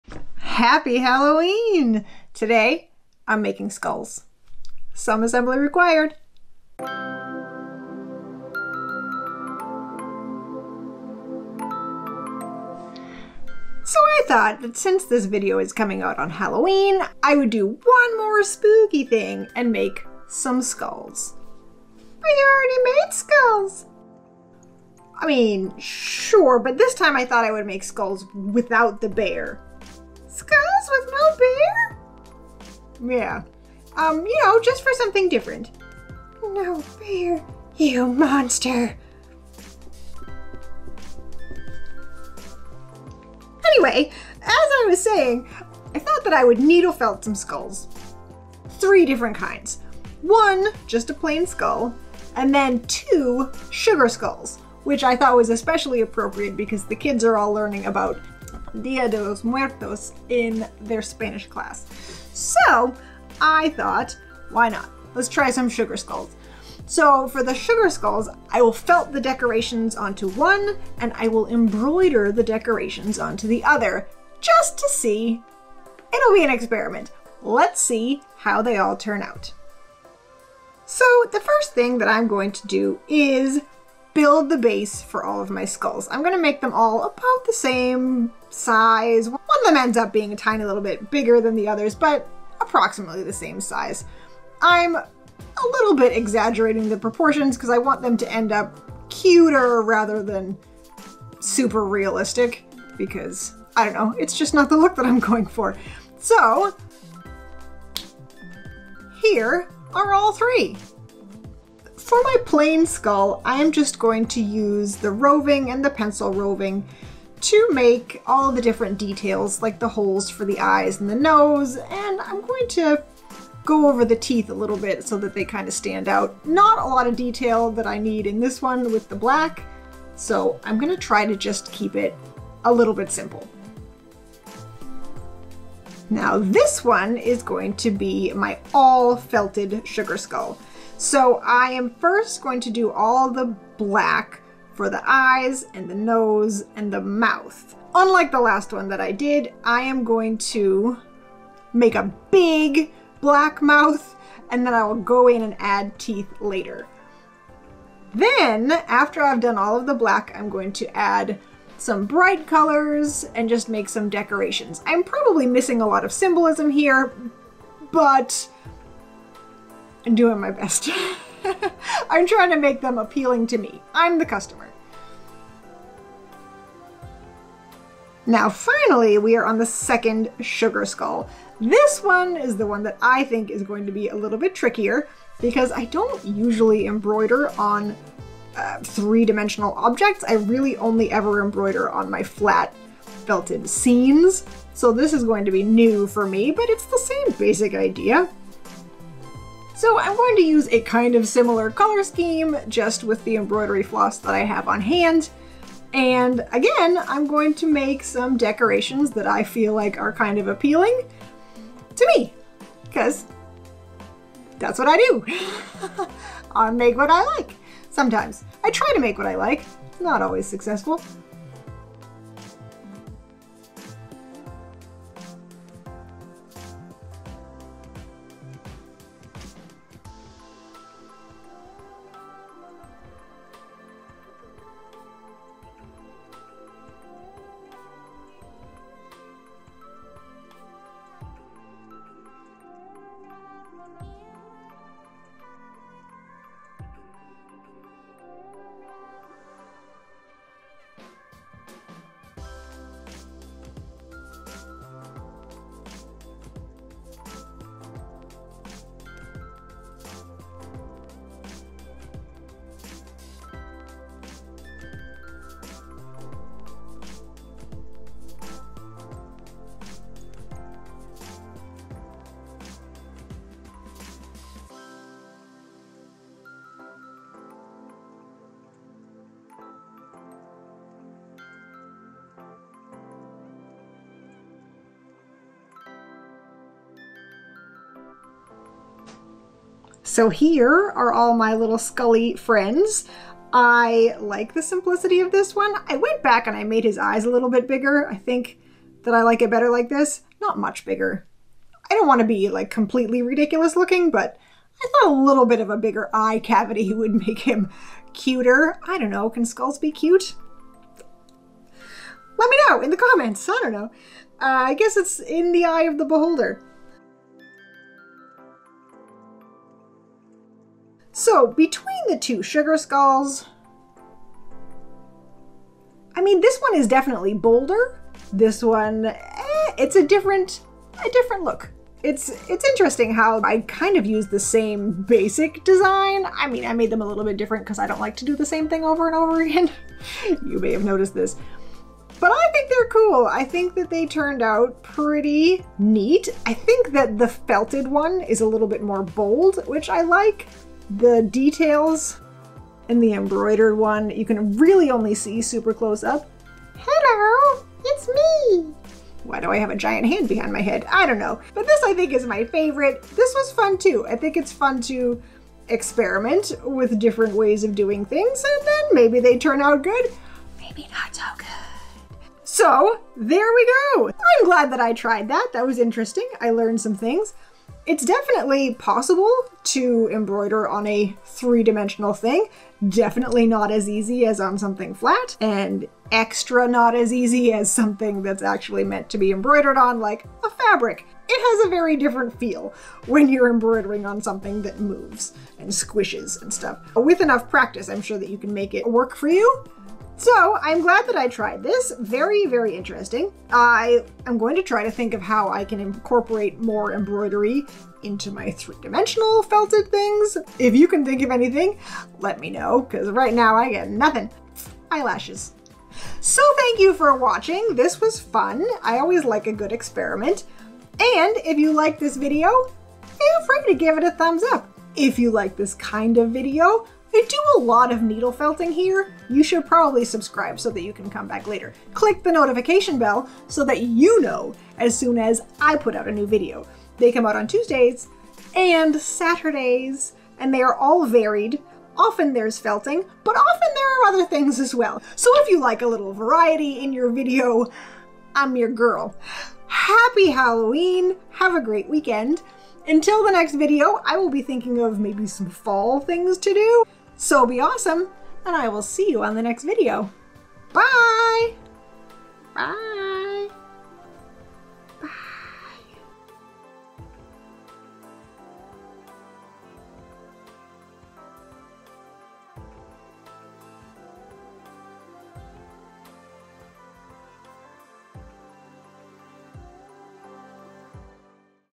Needle Felted Skull Decorations: Three Ways! (One with embroidery!)
In this video, I needle felt three different skulls for Halloween and the day of the dead. One skull is just a plain normal skull, but the other two are my interpretations of Calaveras, or sugar skulls. One sugar skull is done entirely with needle felting, the other combines felting with embroidering. This is my first time embroidering on a three dimensional needle felted object, but I've done a lot of embroidery on flat felted landscapes. Both are fun! The only things you have to keep in mind with your 3d objects are that the embroidery might squish things that didn't need squishing and that you'll have to hide your knots somewhere. I just hid the knots with a little bit of extra skull in the back, which also solved a lot of my squishing problems! I'm not sure that I would use embroidery quite as much on 3d felted objects, but it's good to know it's possible. Happy Halloween and Dia de los Muertos!